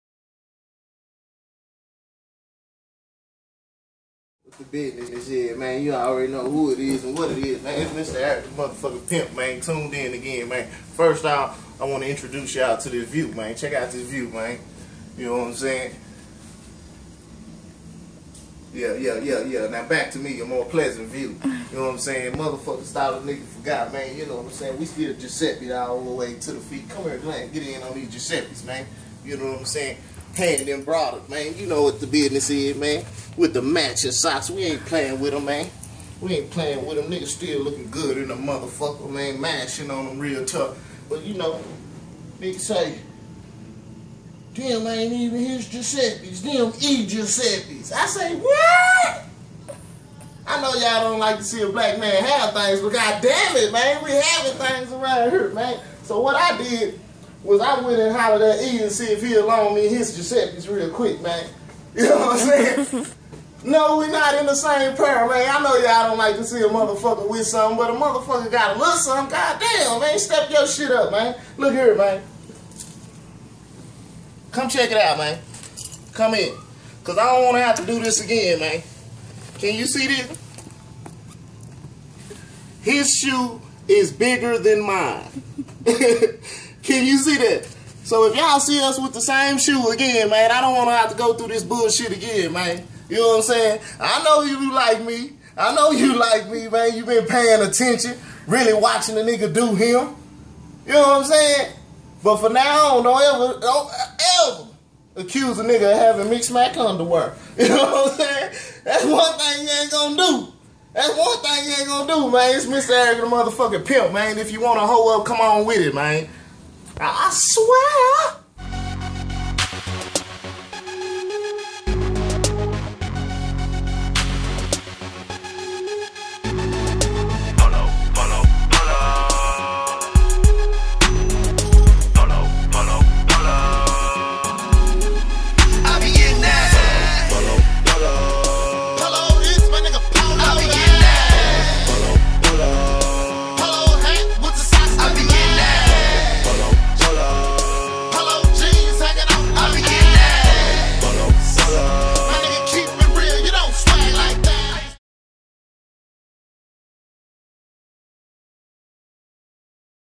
Mr Arrogant in Vegas
Mr Arrogant takes a couple minutes out his day to address a few jealous fans iSWEAR...